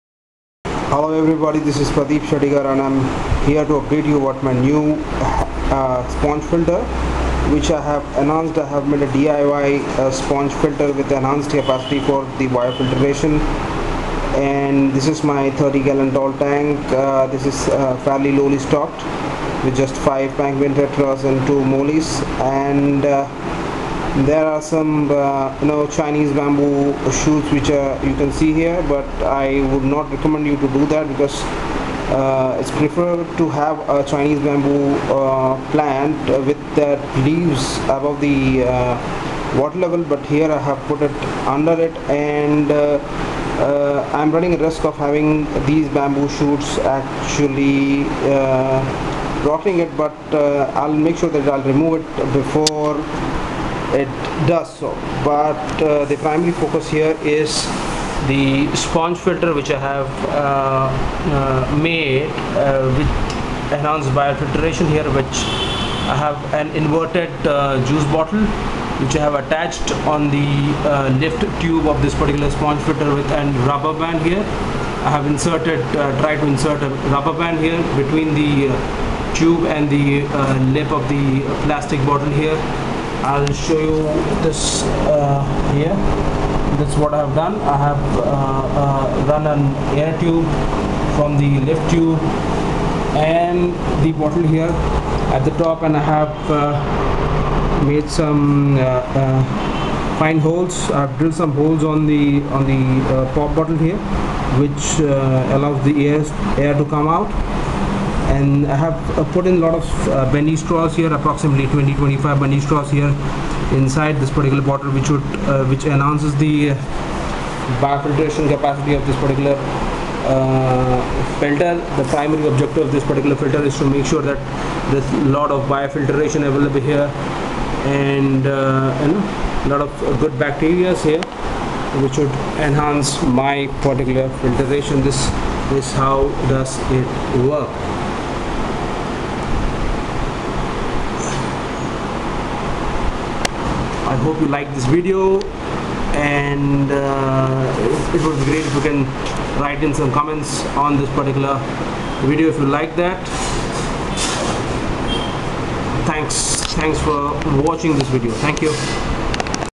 DIY HYBRID SPONGE AND MOVING MEDIA AQUARIUM FILTER
This is an DIY Bio-Sponge filter were in with zero additional con we can enhance the bio filtration capacity of an simple sponge filter for initial cycling of an new tank or use it as and additional bio filtration medium for overstocked fish tank

In this particular video i have attached and simple sponge filter with an inverted juice bottle with lot of chopped bendy straws in to it, like an moving bed filter to greatly enhance the bio filteration capacity of the simple sponge filter